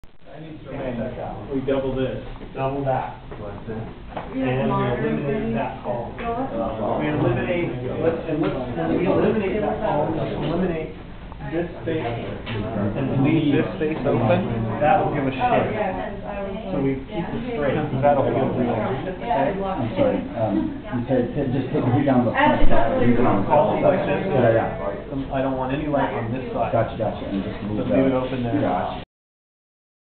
SELFLESS - Behind The Scenes
Cinematographer, Kevin Fletcher, works with the crew to light a night shot in the Louisa.